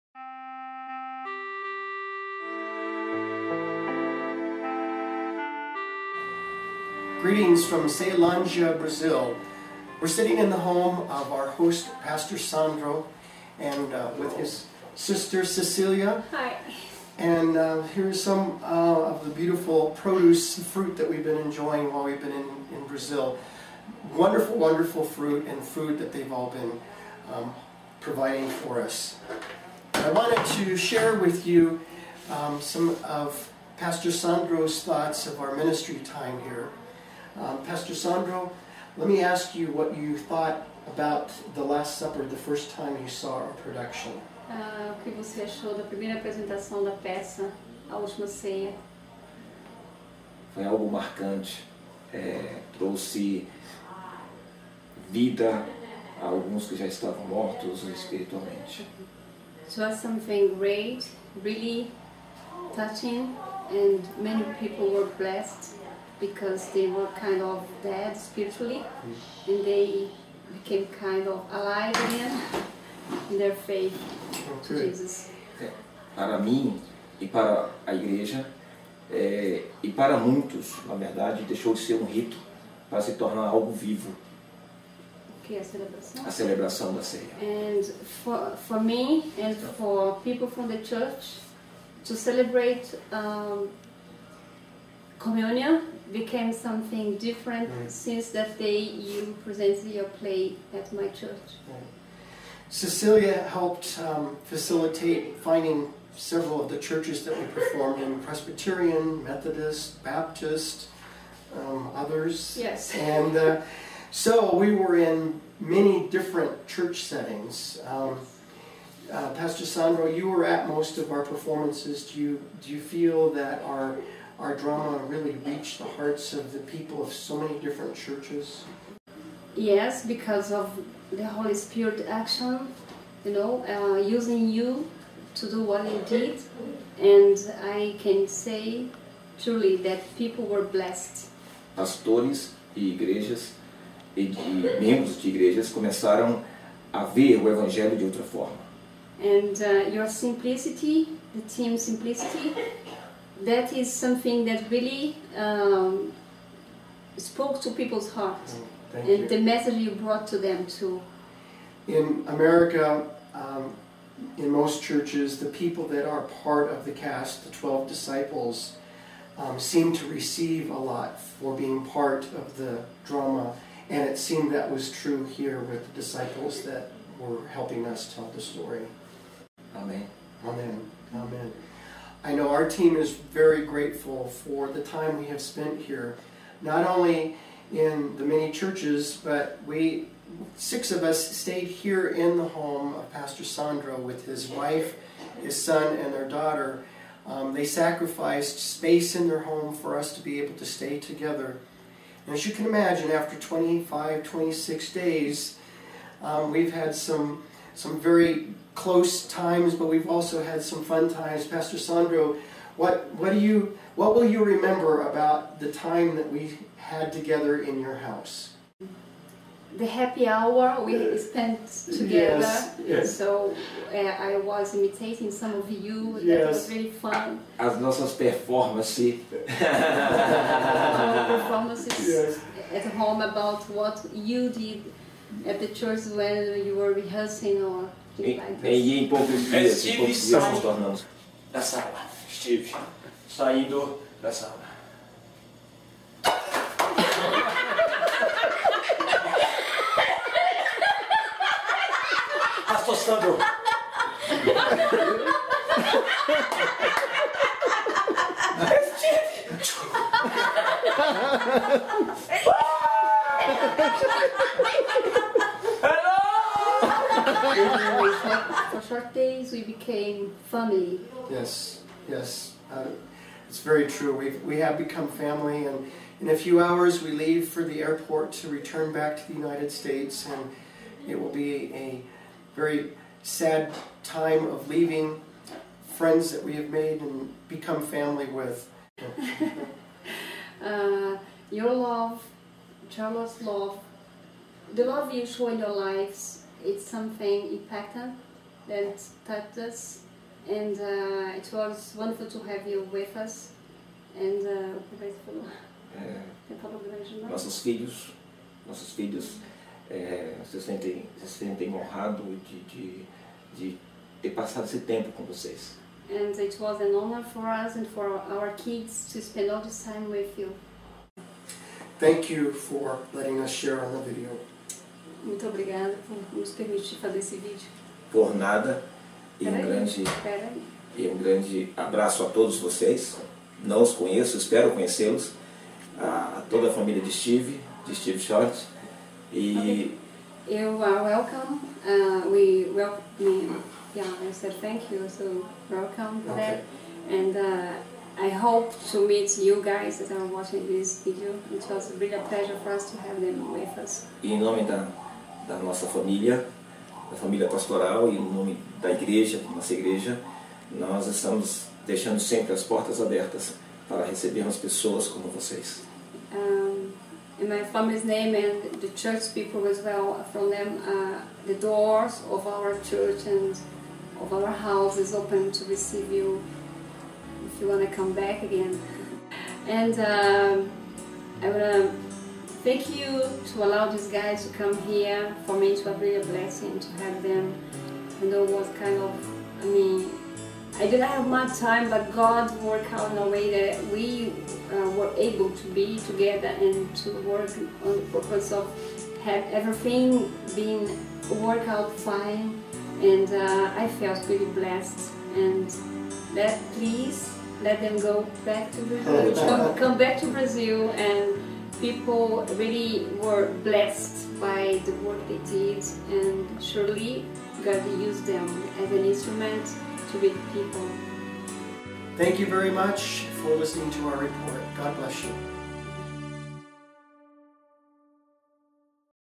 Brazil 2010 Outreach Interview
This is an interview with Pastor Sandro, our host. He comments on his observation of our ministry in Brazil sharing THE LAST SUPPER drama in Portuguese.